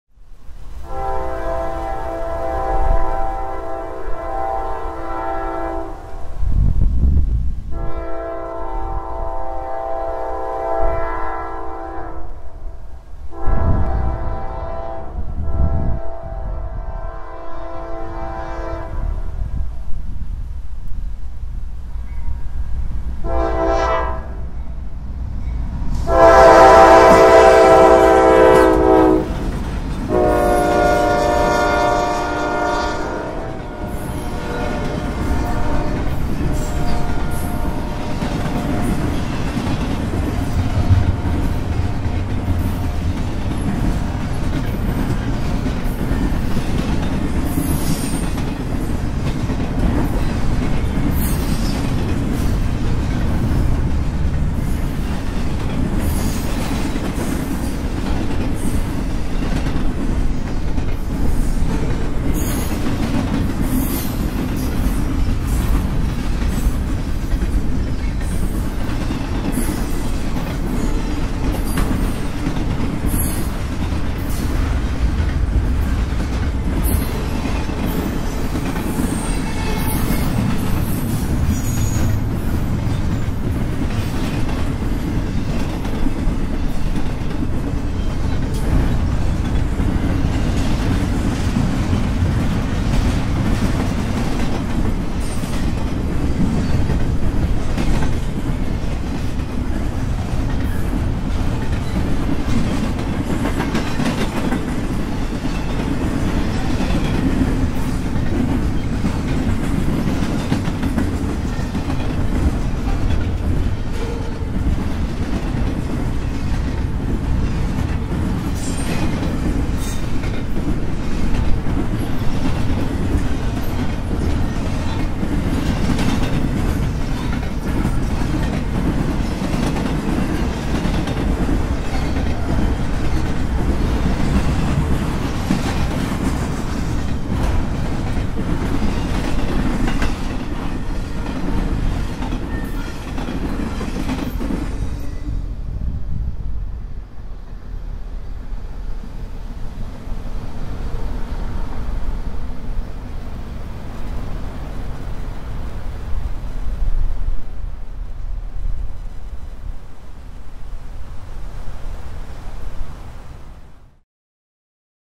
ES40DC Pair & Friendly Engineer on CSX - Plenty of Horn Sound
A CSX container train is southbound in Athens, Alabama.

The locomotives are CSX 5266 and CSX 5249.